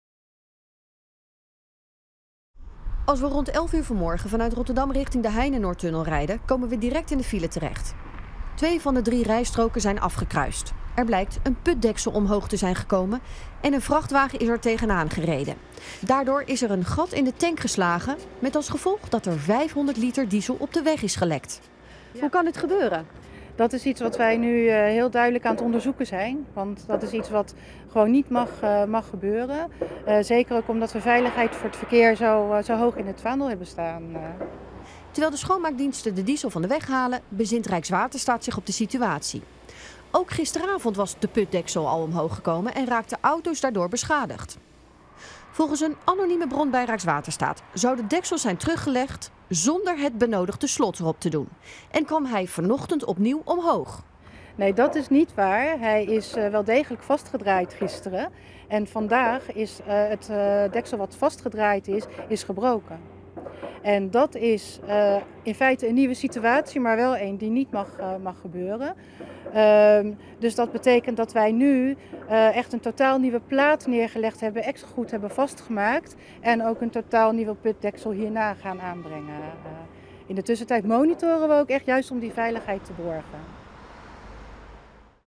Putdeksel hindert verkeer op de A29
De Heinenoordtunnel ten zuiden van Rotterdam heeft woensdag lange tijd last gehad van vertragingen. In de richting Zierikzee moesten rijstroken van de A29 worden afgesloten omdat er een putdeksel omhoog was gekomen.

   Lees meer: Putdeksel hindert verkeer op de A29